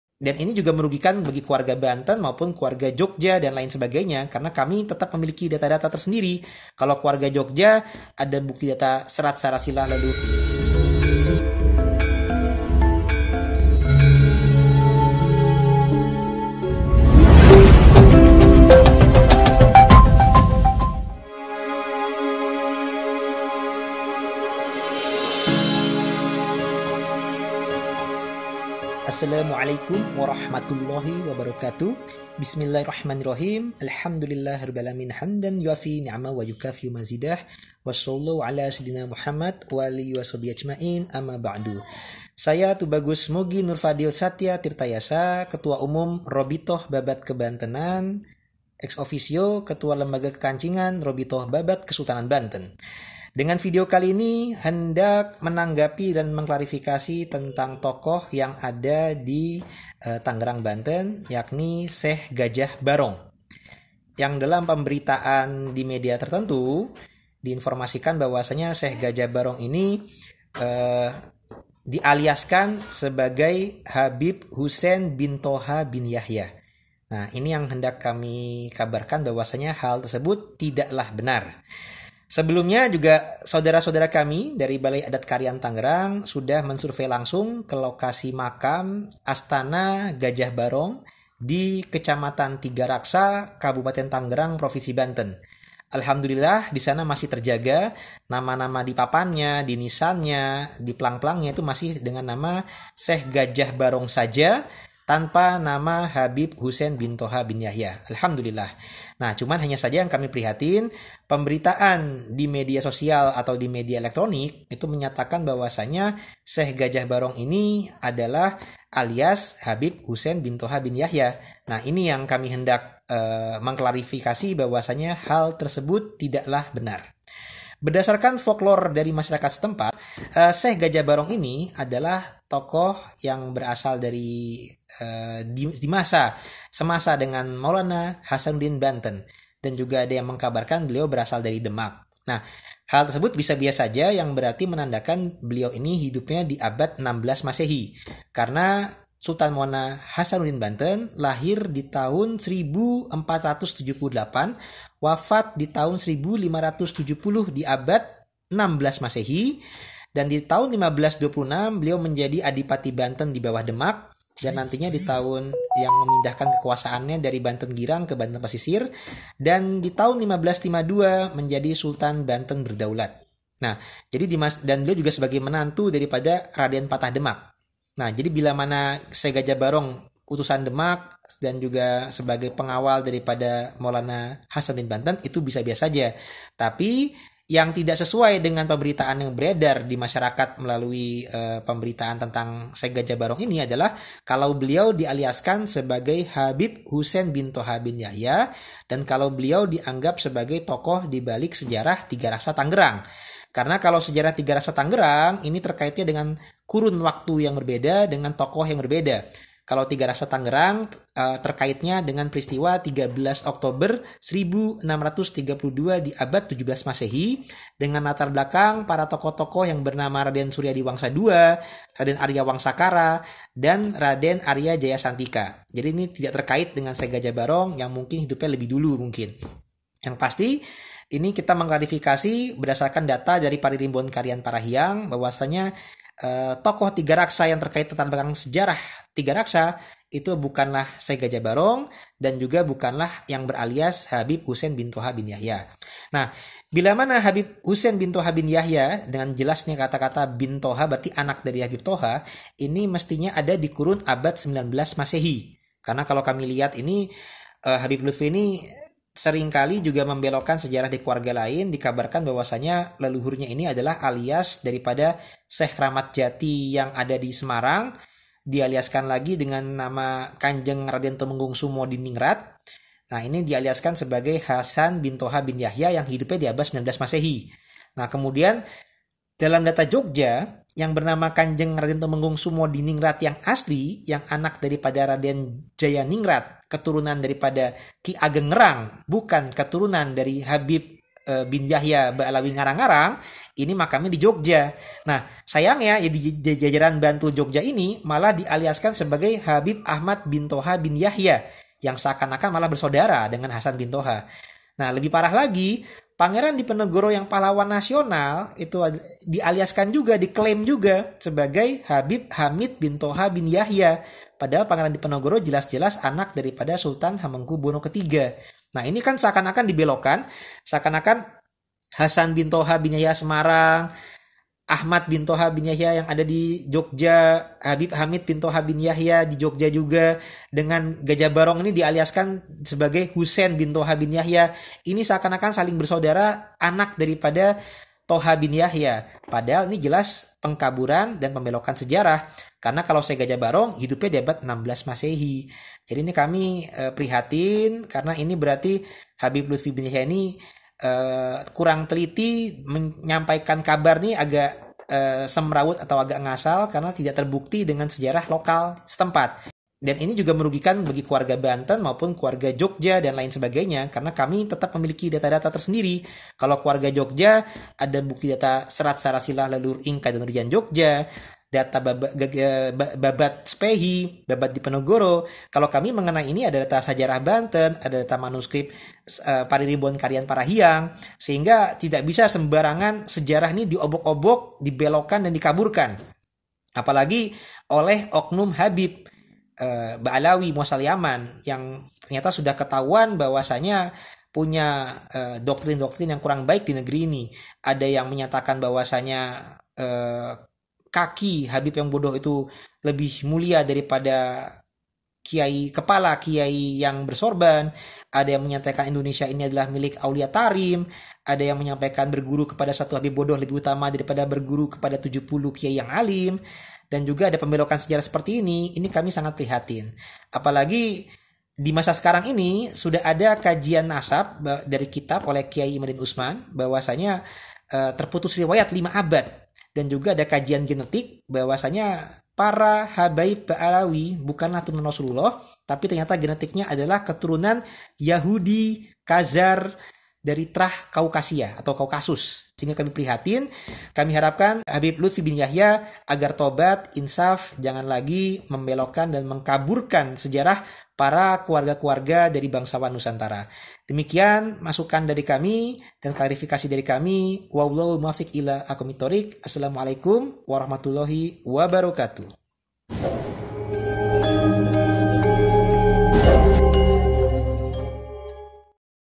Tokoh Muda Banten G3R4M, Begitu Banyak Makam Tokoh DIP4L5UKAN | Gajah Barong
Salam hormat...

Pernyataan Tb Mogi Nurfadhil Satya Tirtayasa (Ketua Rabithah Babad Banten)